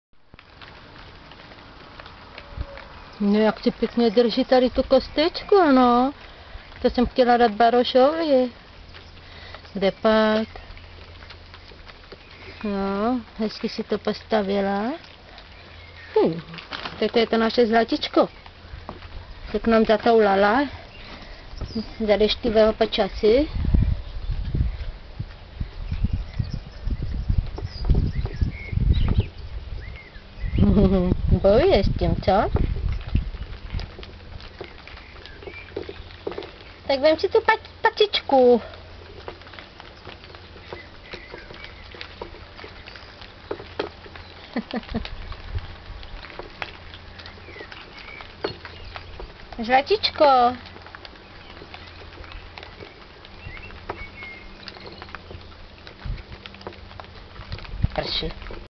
kocka a kost
kosti byly pro psa, ale kocka se na to vrhla